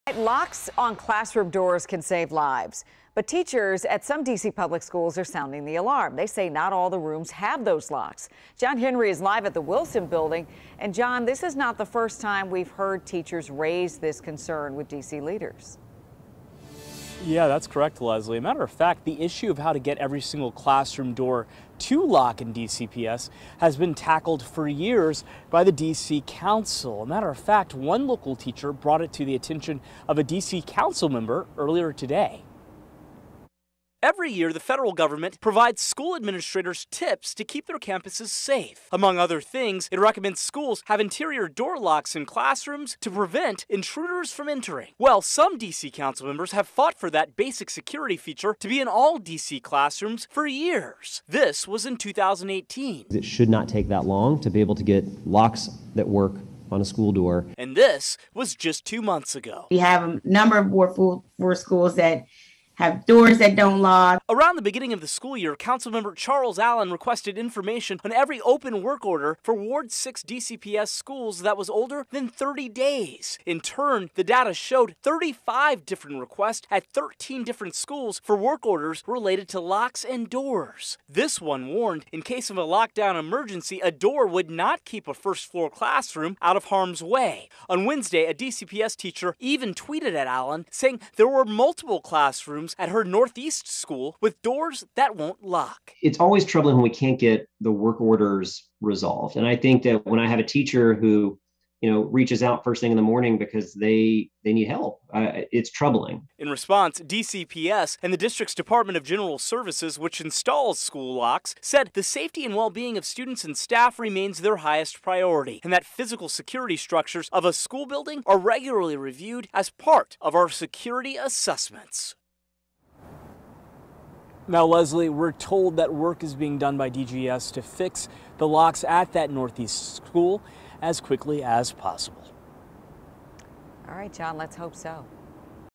DCPS teachers demand better security at schools
Some DC Councilmembers have fought for one basic security feature to be in all DC classrooms for years.